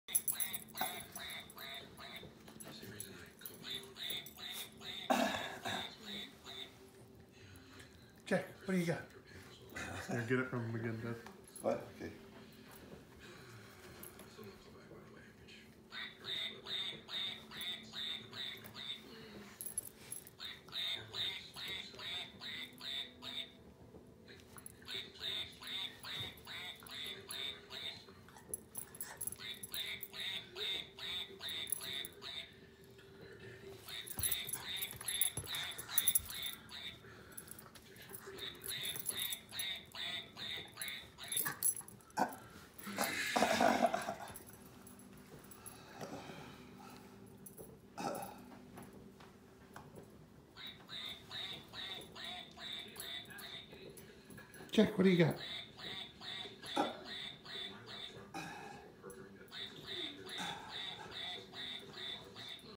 A Boy and his Duck
Jack plays with his pet duck and with Grandpa